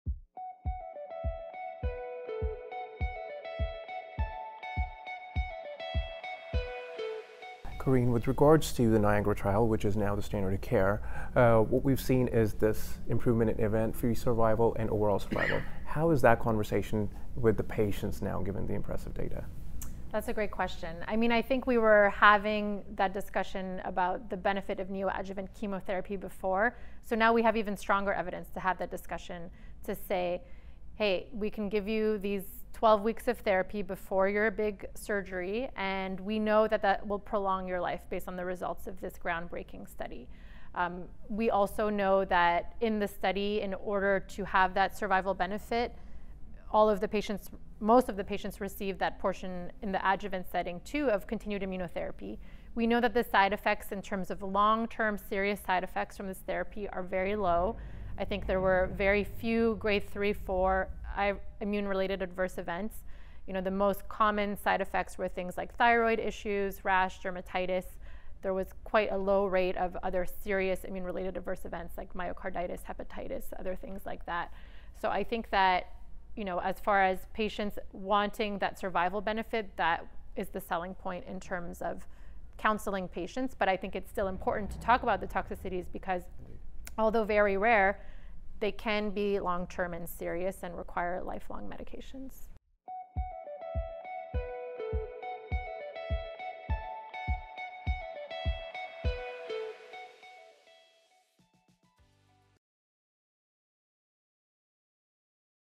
The Oncologist’s Role in Counseling Patients With MIBC Through Complex Treatment Decisions
In this episode, we dived into the groundbreaking findings from the NIAGARA trial, which has now become the standard of care in cancer treatment. Here Dr. Tawagi discussed the significant improvements in event-free survival and overall survival that have emerged from this study.

We explored the benefits of neoadjuvant chemotherapy, highlighting how a 12-week therapy regimen before surgery can prolong life. Dr. Tawagi also addressed the importance of continued immunotherapy in the adjuvant setting and reassured listeners about the low incidence of serious long-term side effects associated with this treatment.

While the survival benefits are compelling, we also emphasized the need to discuss potential toxicities, even if they are rare. Tune in for an insightful conversation that balances hope with the realities of cancer treatment.